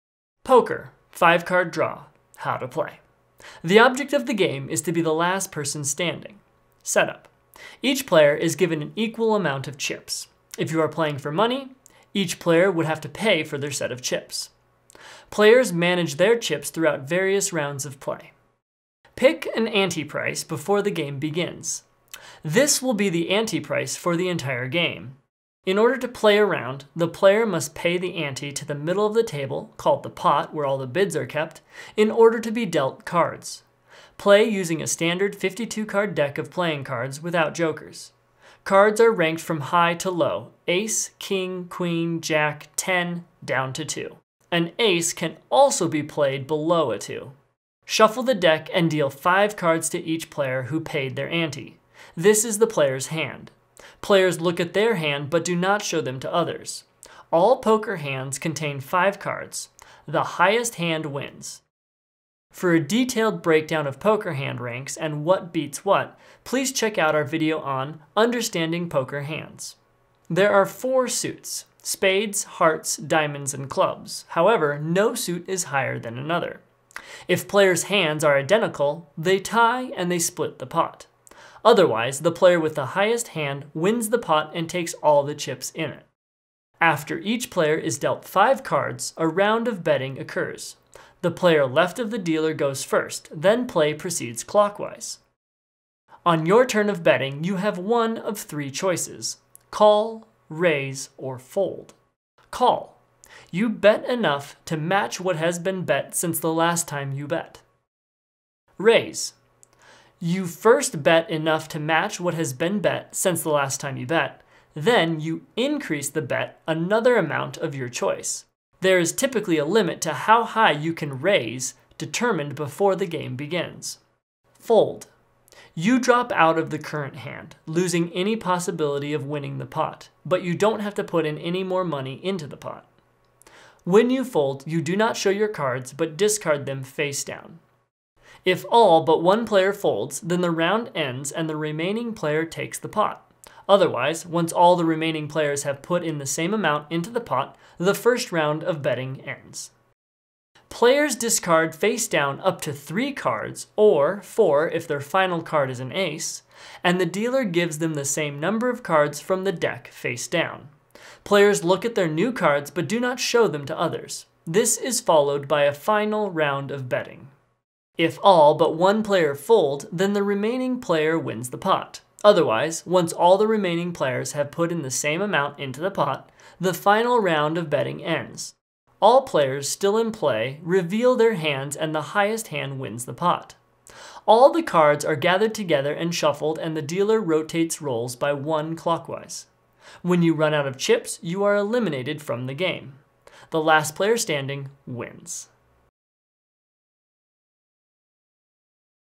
How to play Poker - 5 Card Draw
Learn the rules to the card game Poker 5 card draw quickly and concisely - This visually rich video has no distractions, just the rules. If you need help Understanding Poker Hands, check out this video

RULES:

The object of the game is to be the last person standing. Setup. Each player given an equal amount of chips, if you are playing for money, each player would have to pay for their set of chips. Players manage their chips throughout various rounds of play.

Pick an ante price before the game begins. This will be the ante price for the entire game. In order to play a round, a player must pay the ante to the middle of the table, called the pot where all the bids are kept, in order to be dealt cards. Play using a standard 52 card deck of playing cards, without jokers. Cards are ranked from high to low: Ace, King, Queen, Jack, 10 down to 2. An Ace can also be played below a 2.

Shuffle the deck and deal 5 cards to each player who paid their ante. This is the player’s hand. Players look at their hand but do not show them to others. All poker hands contain five cards, the highest hand wins. For a detailed breakdown of poker hand ranks and what beats what, please check out our video on “Understanding Poker Hands”.

There are four suits: spades, hearts, diamonds and clubs; however, no suit is higher than another. If player’s hands are identical, they tie and you split the pot. Otherwise the player with the highest hand wins the pot and takes all the chips in it.

After each player is dealt five cards, a round of betting occures. The player left of the dealer goes first, then play proceeds clockwise. On your turn of betting you have 1 of 3 choices: Call, Raise, or Fold.

Call: you bet enough to match what has been bet since the last time you bet.

Raise: you first bet enough to match what has been bet since the last time you bet, then you increase the bet another amount of your choice. There is typically a limit to how high you can raise determined before the game begins.

Fold: you drop out of the current hand, losing any possibility of winning the pot, but you don't have to put any more money into the pot. When you fold, you do not show your cards, but discard them facedown.

If all but one player folds, then the round ends and the remaining player takes the pot. Otherwise, once all the remaining players have put in the same amount into the pot, the first round of betting ends.

Players discard facedown up to 3 cards, or 4 if their final card is an ace, and the dealer gives them the same number of cards from the deck facedown. Players look at their new cards but do not show them to others. This is followed by a final round of betting.

If all but 1 player fold, then the remaining player wins the pot. Otherwise, once all the remaining players have put in the same amount into the pot, the final round of betting ends. All players still in play, players reveal their hands and the highest hand wins the pot.

All the cards are gathered together and shuffled and the dealer rotates roles by one clockwise. When you run out of chips you are eliminated from the game. The last player standing wins.